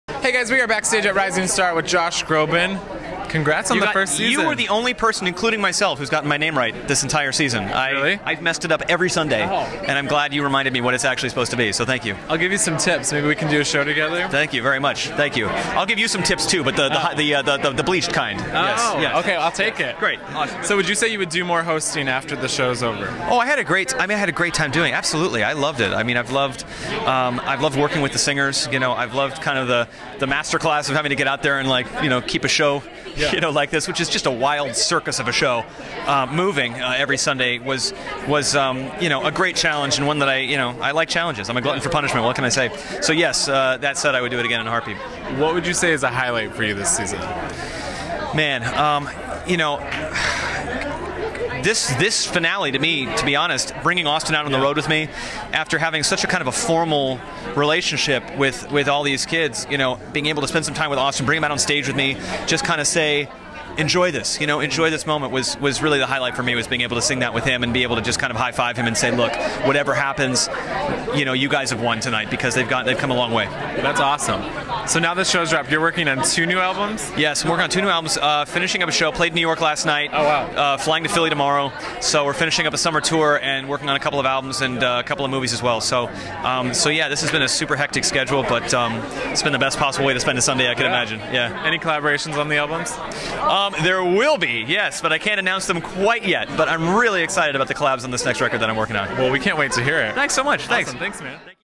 Rising Star Finale Red Carpet with Josh Groban I AfterBuzz TV Interview
Follow Jason on Instagram and Twitter: @JasonIkeler